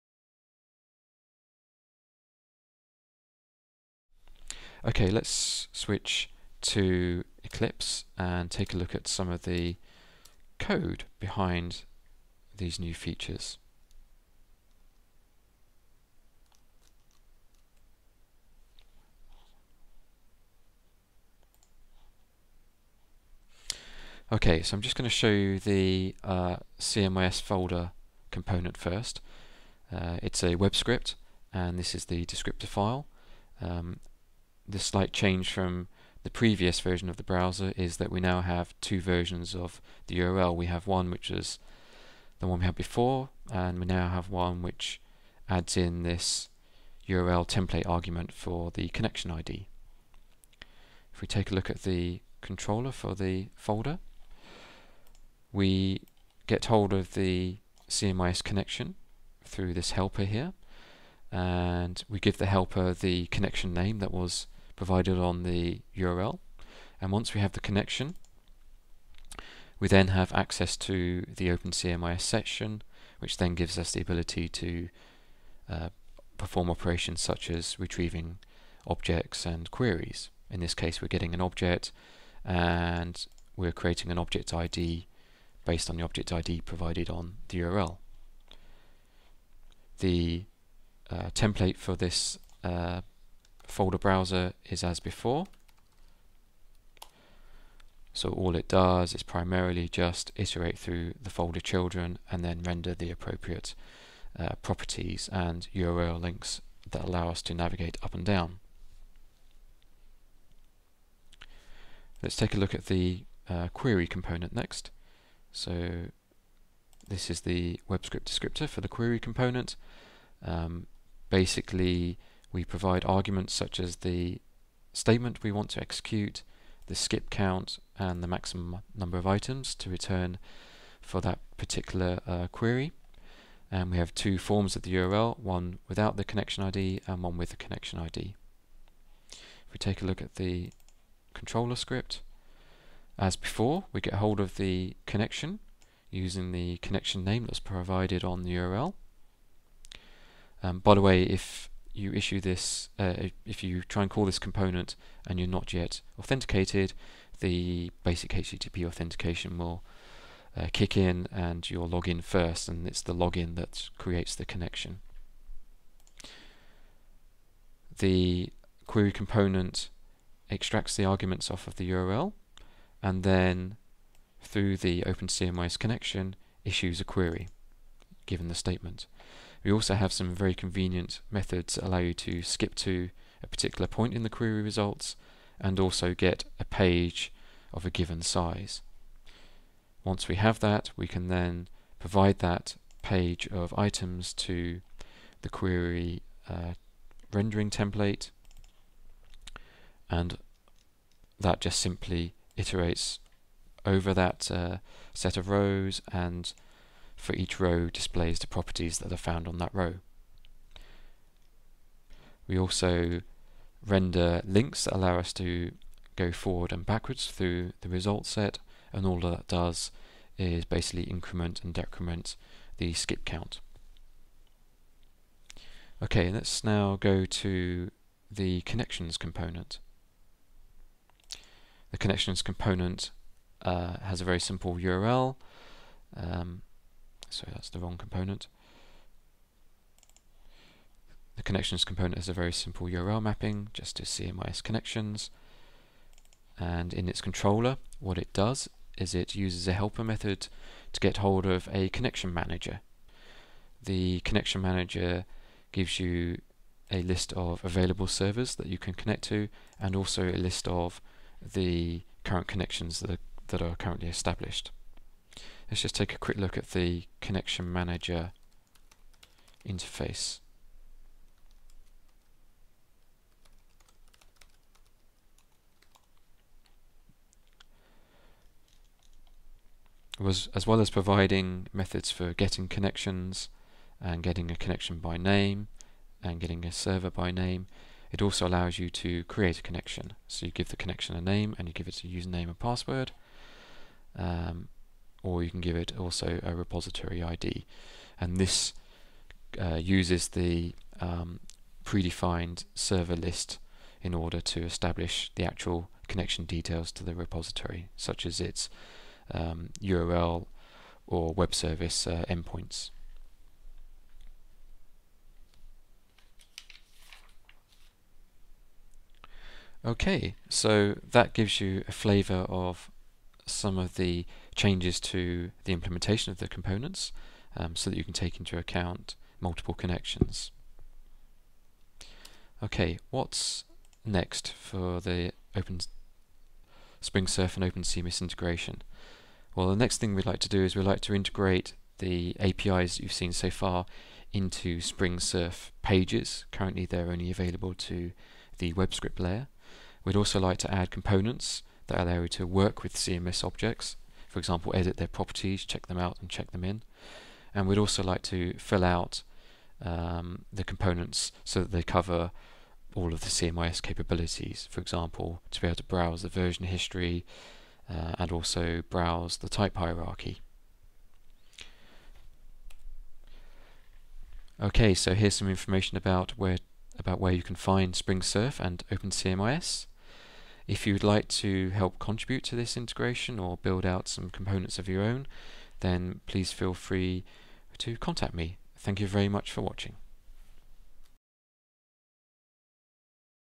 Spring Surf and OpenCMIS Integration Pt 2B
Demonstration of CMIS Browser web application built using Spring Surf and OpenCMIS platform. Shows new property, relationship, rendition and query components. Connects to multiple CMIS repositories at once with demo of search across Alfresco and IBM.